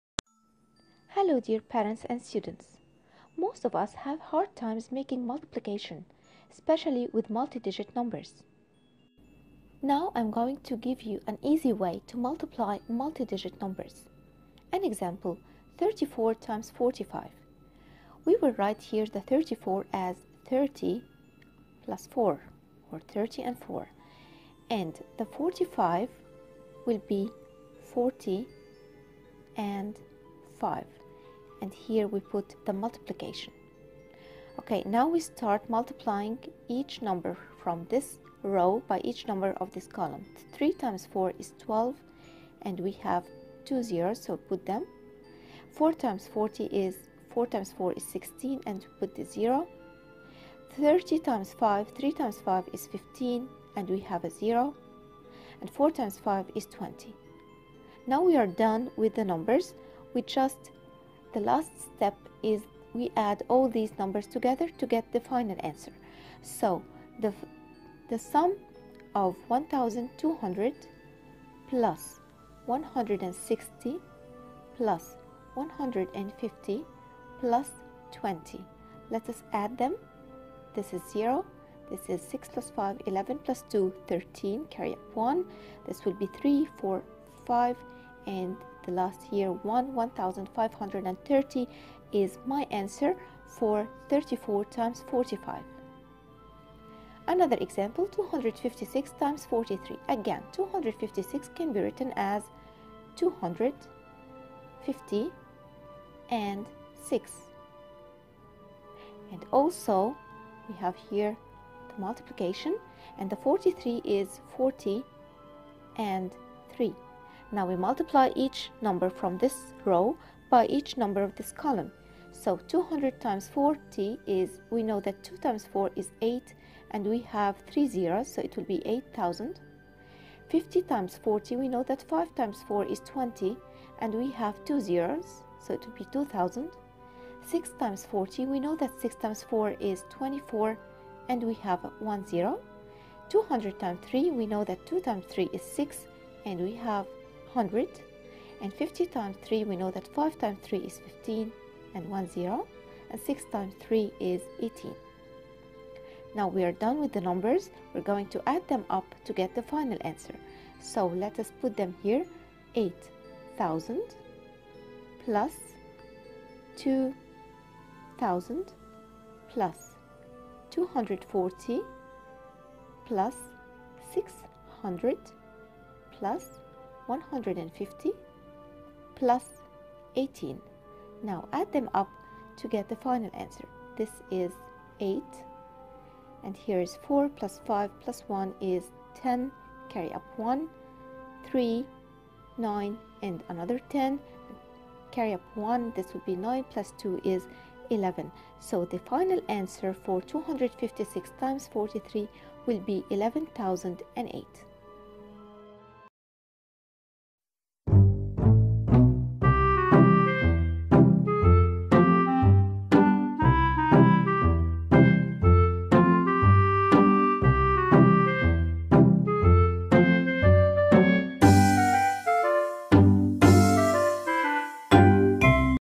The Easiest way to Multiply Multi-Digit Numbers
In this video, we will learn multi-digit multiplication by using the expanded form of numbers.